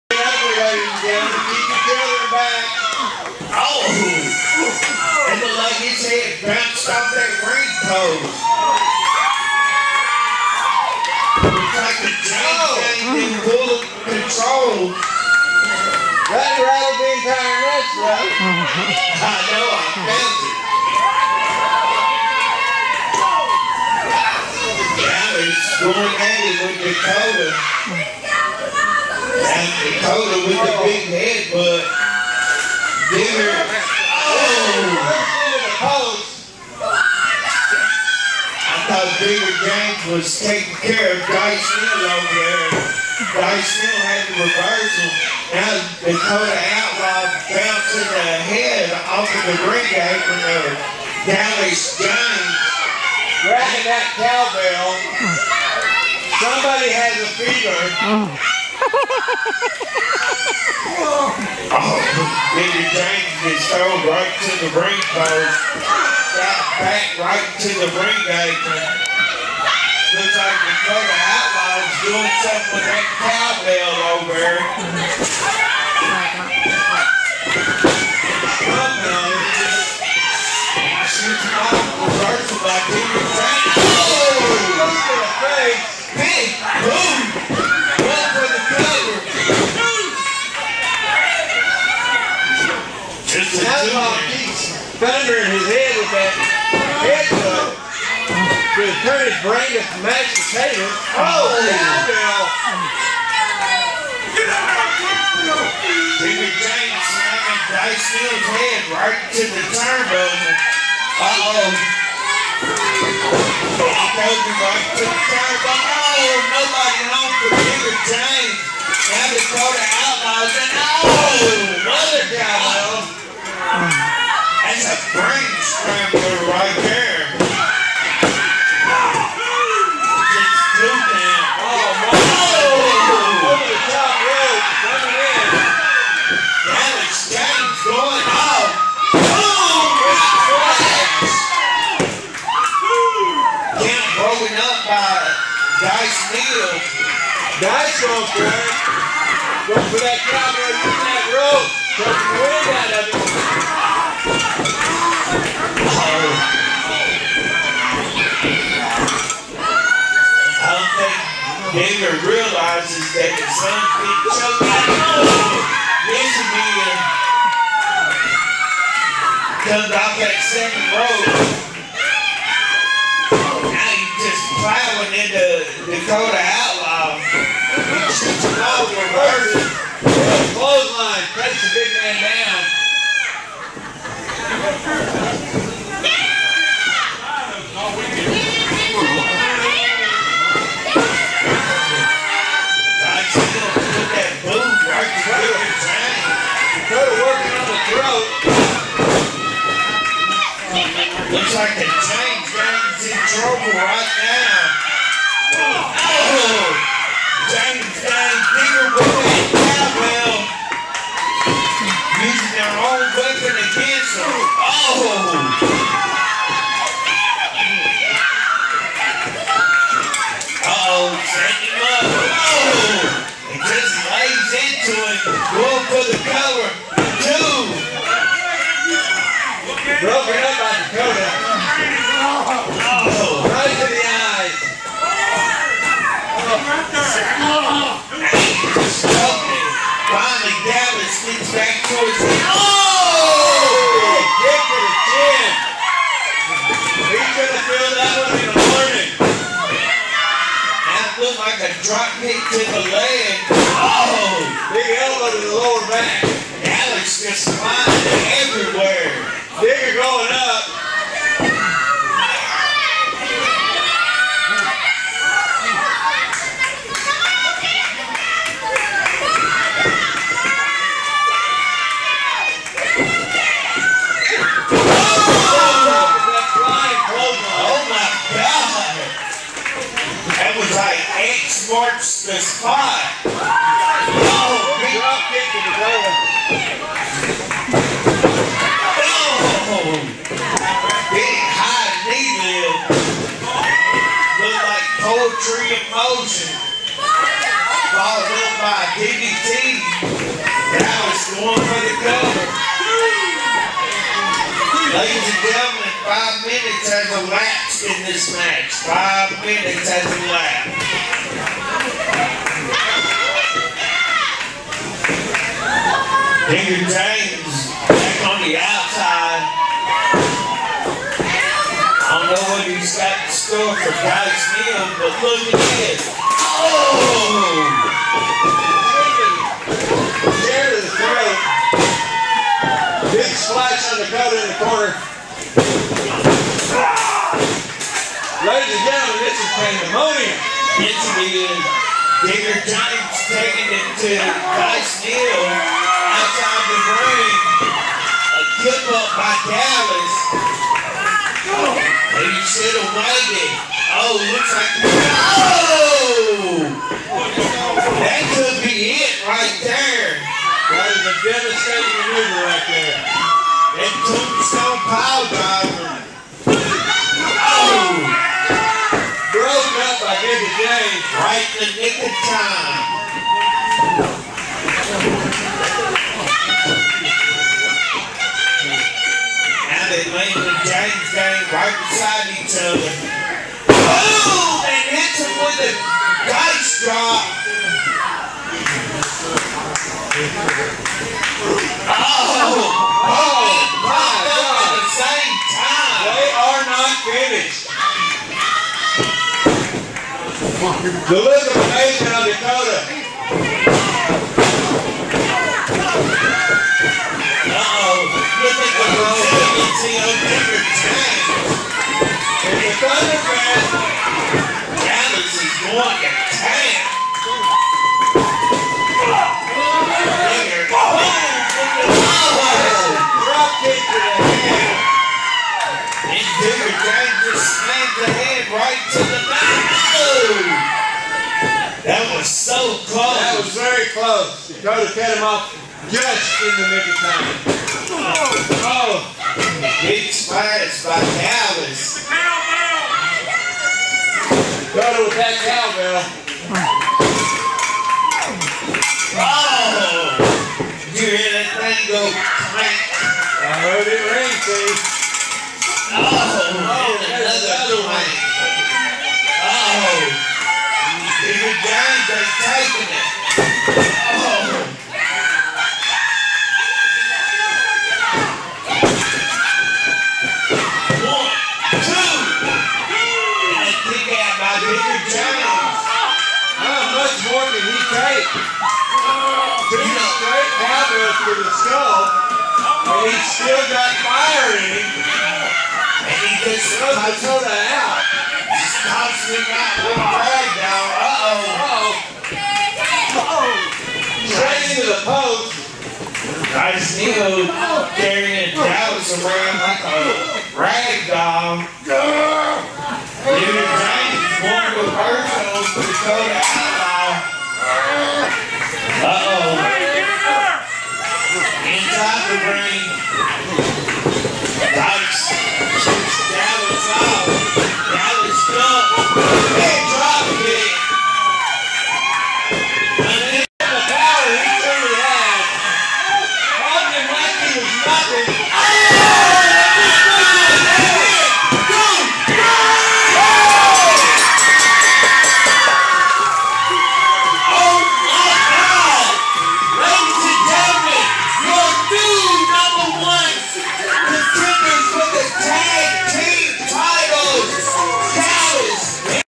TWA - Dakota Outlaw & Dice Neal vs James Gang - August 8, 2020
Night of the Butcher - Tag Team #1 Contender Match - August 1, 2020
** First minute of the Match is Tilted Sideways